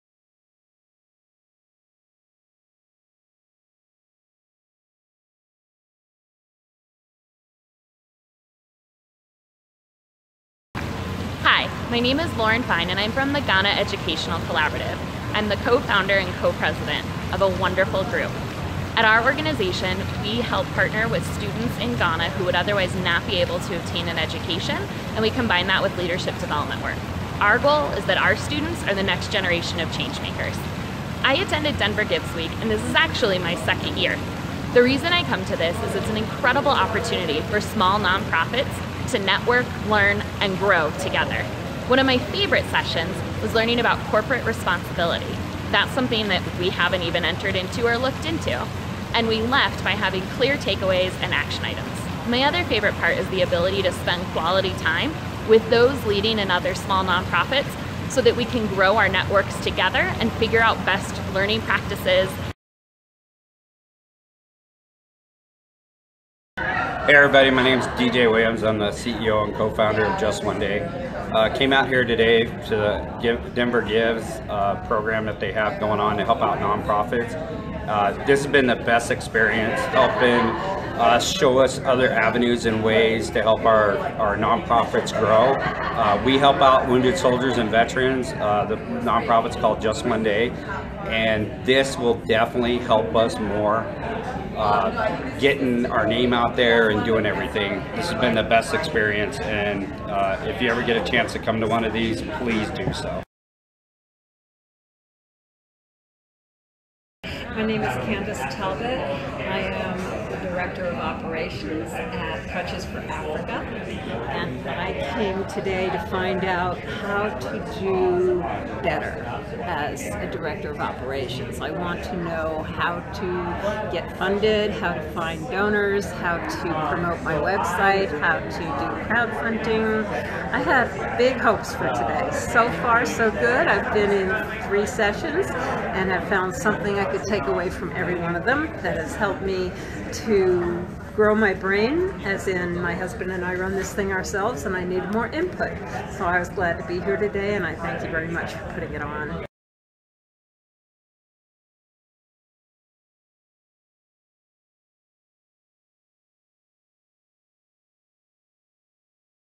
DoingGood Week(end) Testimonial Video
Three small 501c3 nonprofits give their feedback about why they attended DoingGood Week (2016) and what they got out of it for their organizations.